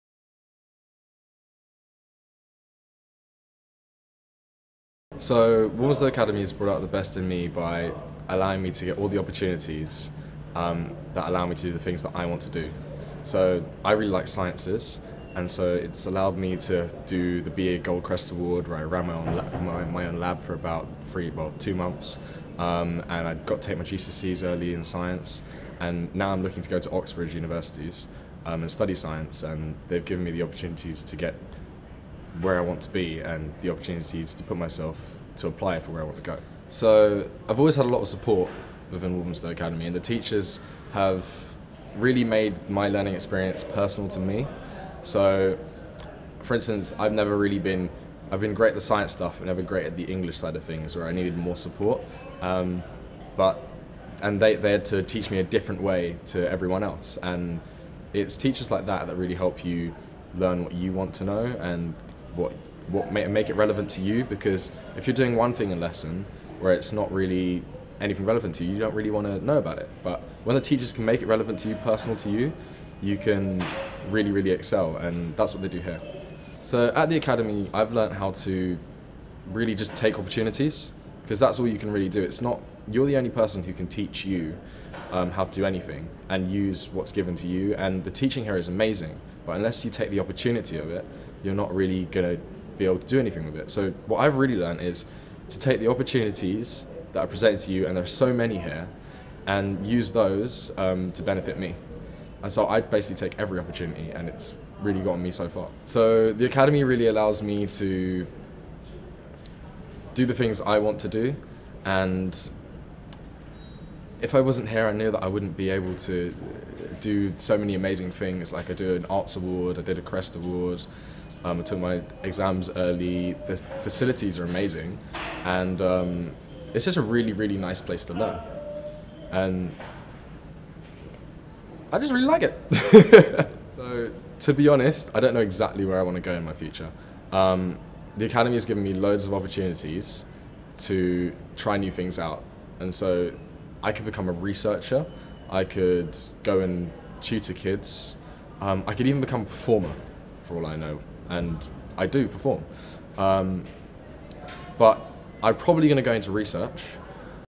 Bringing out the best in me.
Interviews with Walthamstow Academy students and how the best has been bought out in them.